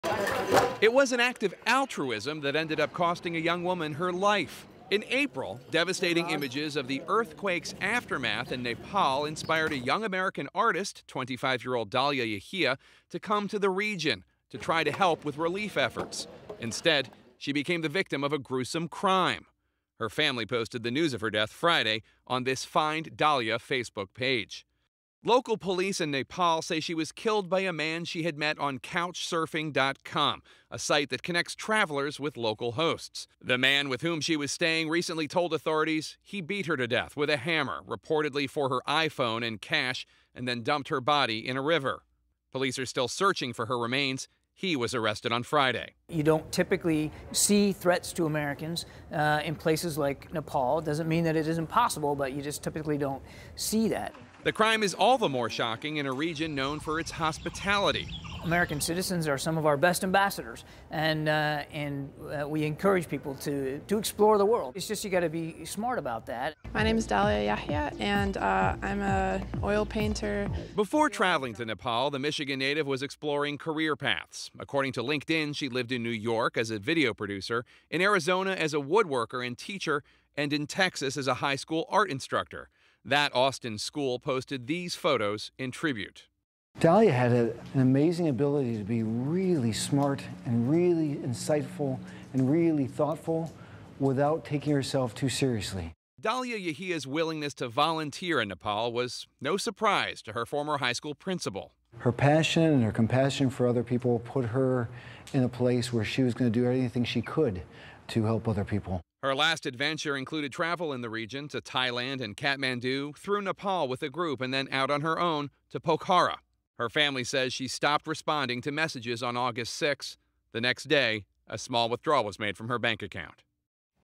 American artist beaten to death in Nepal
Family and friends mourn the loss of Dahlia Yehia, who traveled to Nepal to assist in relief efforts following the devastating earthquake earlier this year. CNN's Jake Tapper reports.